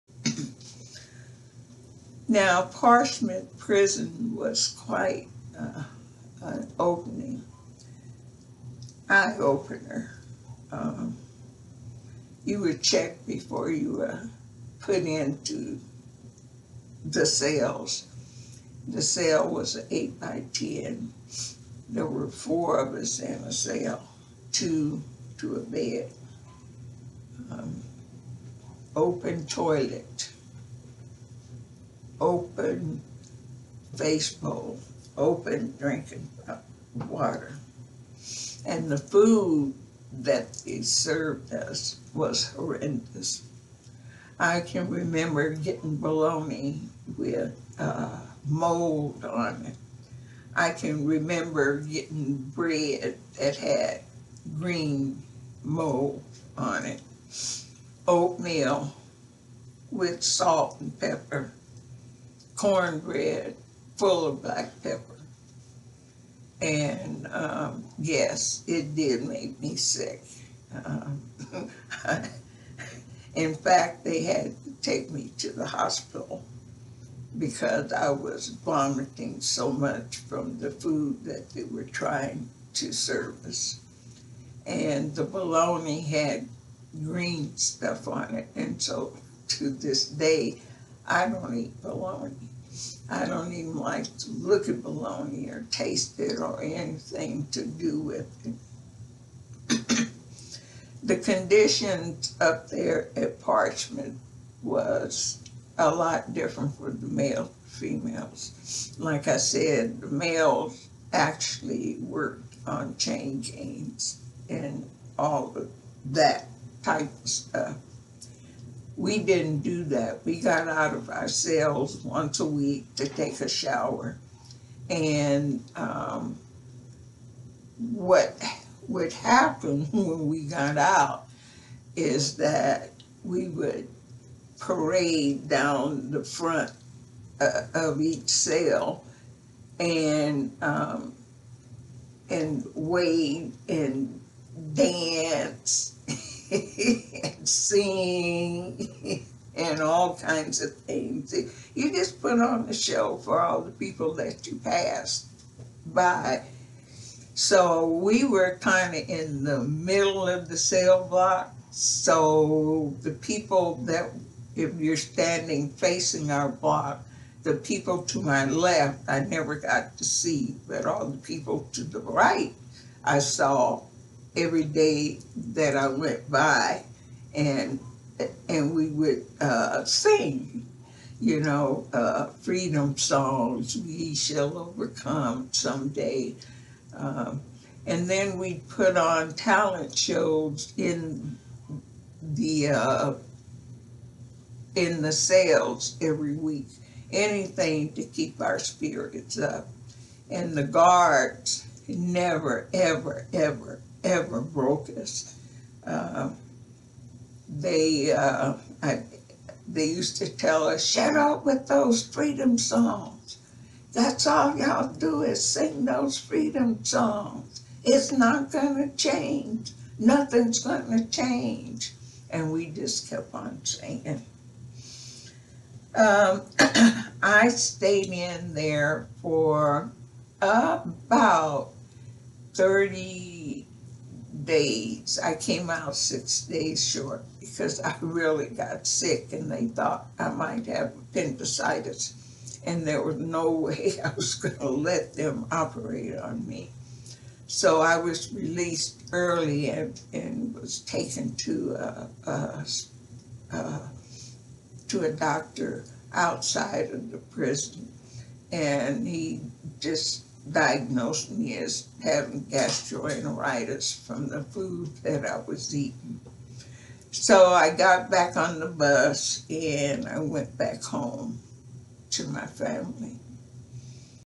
Patricia Dilworth, pt 4 - Freedom Rider
Freedom Rider Patricia Dilworth talks about her incarceration in Parchman Penitentiary, the conditions at the prison, singing Freedom Songs, and being released early due to an illness caused by eating the tainted food served to the Freedom Riders in jail.

Thank you to Reflection Films for sponsoring the transcription of this video.

This video testimony was recorded at the Freedom Rides Museum in Montgomery, AL in 2018.